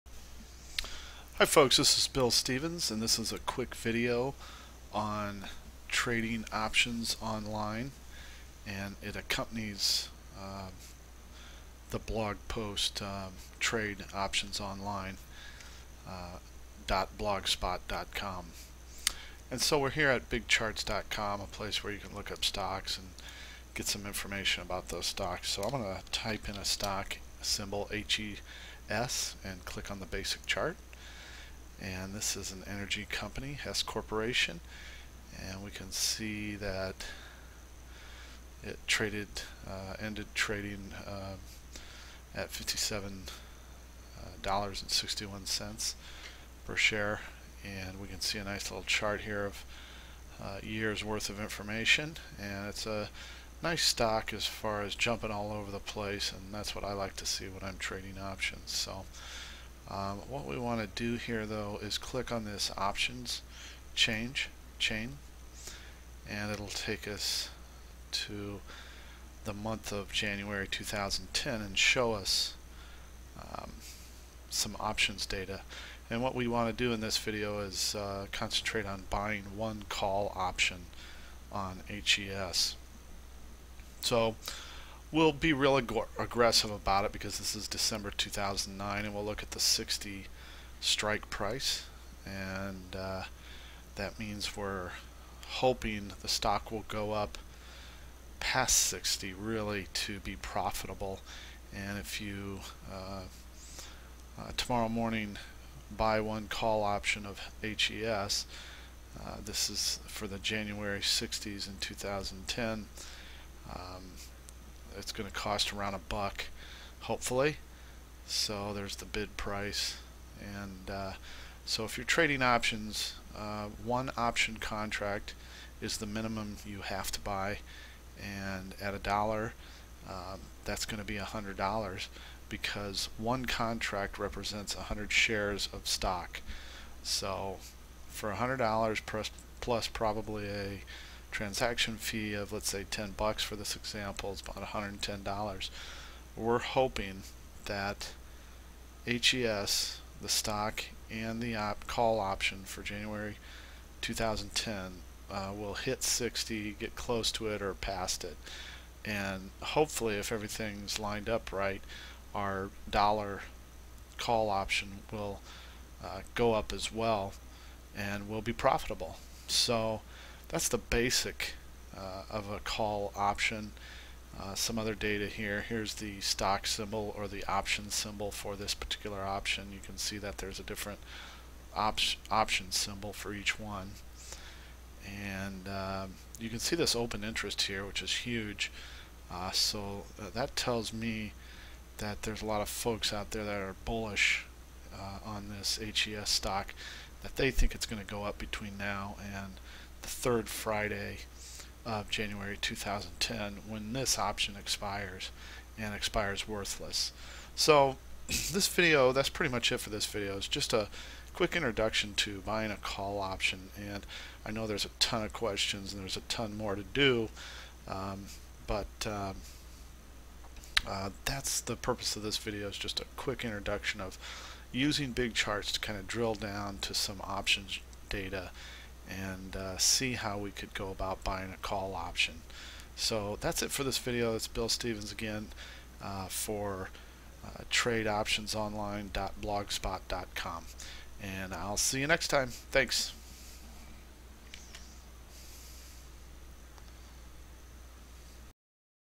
Trade Options Online
- This is a short video on using the website BigCharts.com to look up options information for stocks.